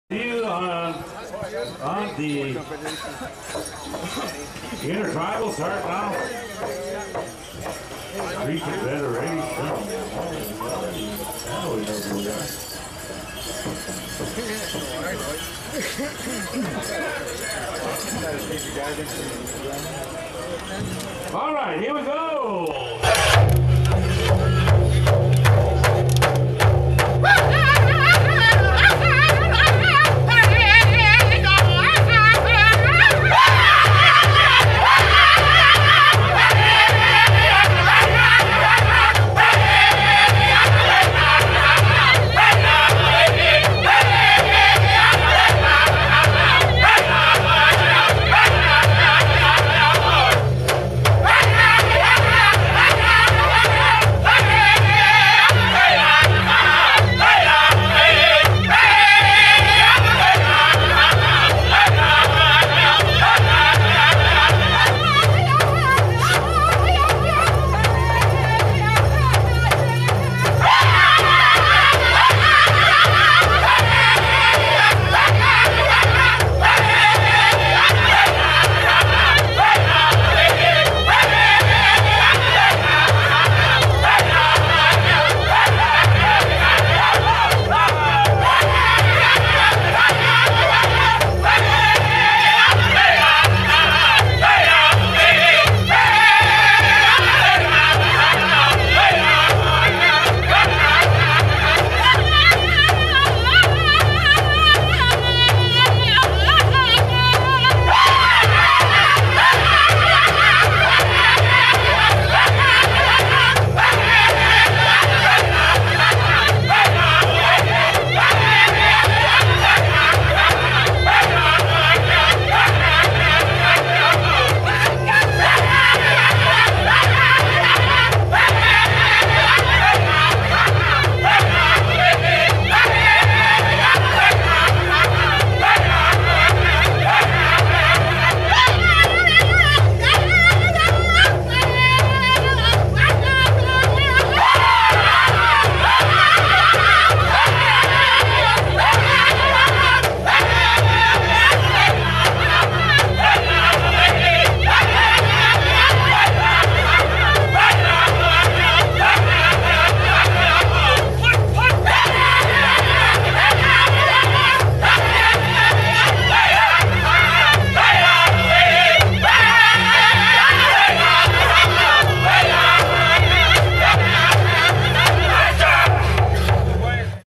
Intertribal Sat Afternoon Cree Confederation 2015 Wild Horse Casino
Wild Horse Casino PowWow 2015 Singing Champs